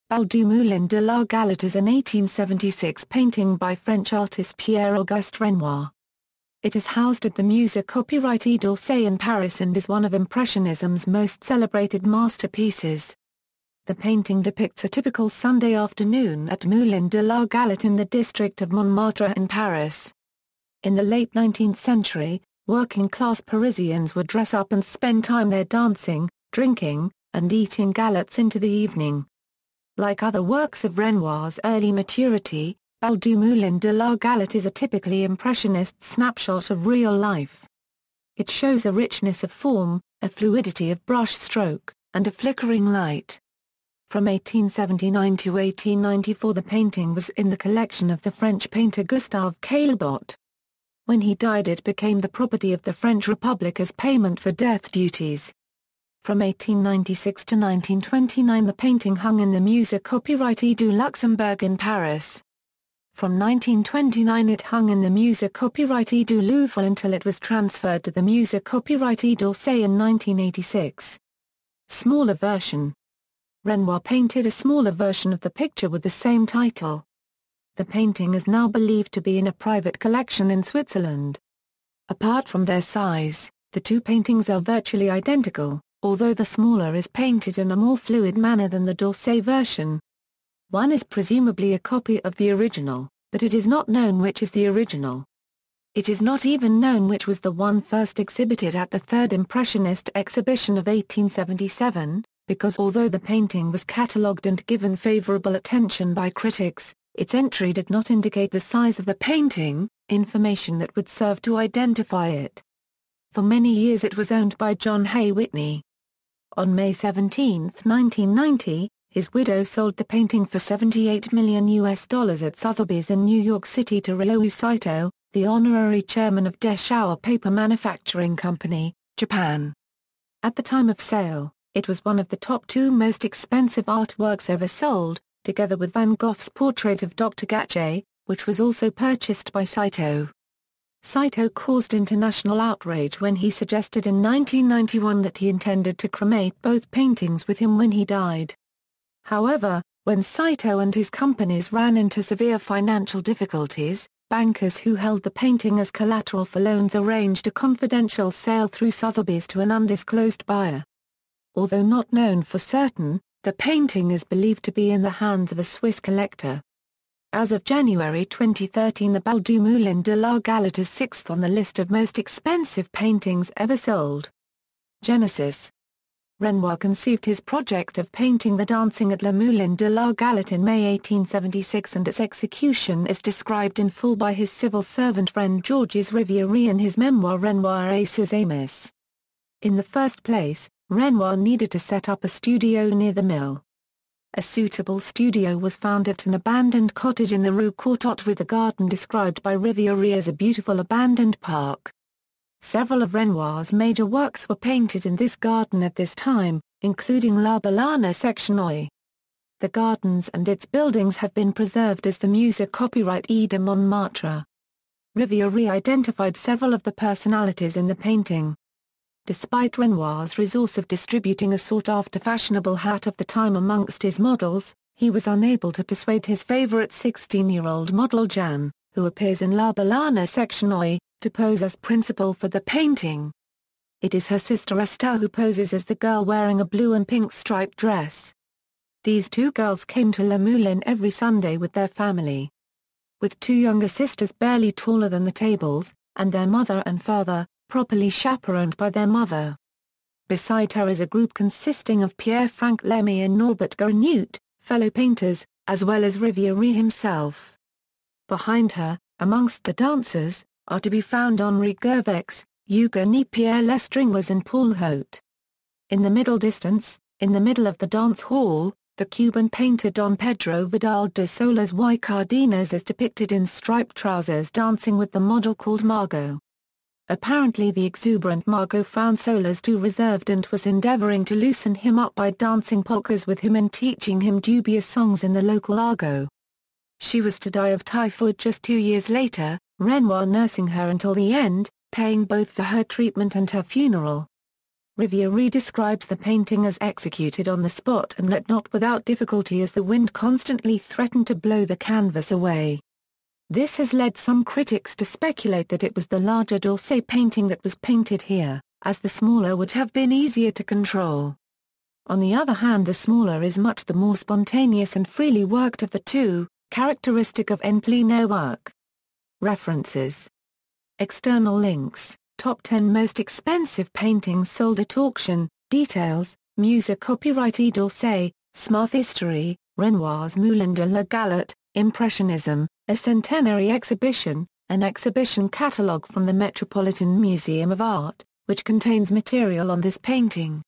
Bal du moulin de la Galette
Bal du moulin de la Galette is an 1876 painting by French artist Pierre-Auguste Renoir. It is housed at the Musée d'Orsay in Paris and is one of Impressionism's most celebrated masterpieces. The painting depicts a typical Sunday afternoon at Moulin de la Galette in the district of Montmartre in Paris. In the late 19th century, working class Parisians would dress up and spend time there dancing, drinking, and eating galettes into the evening.
Like other works of Renoir's early maturity, Bal du moulin de la Galette is a typically Impressionist snapshot of real life. It shows a richness of form, a fluidity of brush stroke, and a flickering light.